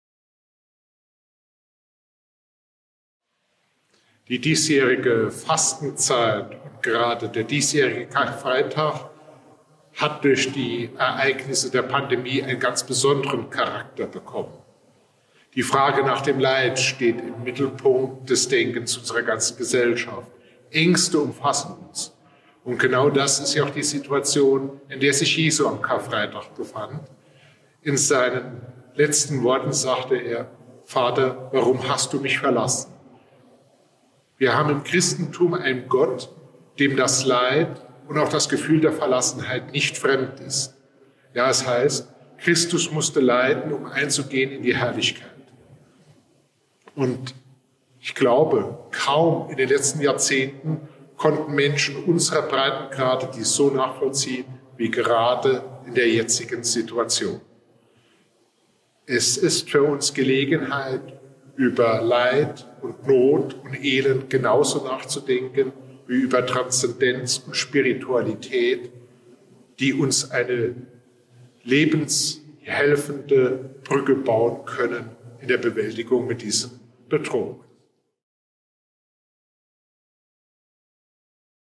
Karfreitag - für einen Liebenden schmerzlichste Situation: Verrat und Verlassenheit.
Die Frage nach dem Leid steht im Mittelpunkt unserer ganzen Gesellschaft. Wir haben im Christentum einen Gott, dem das Leid und auch das Gefühl der Verlassenheit nicht fremd ist.
Frater Wendelinus, Abtei Tholey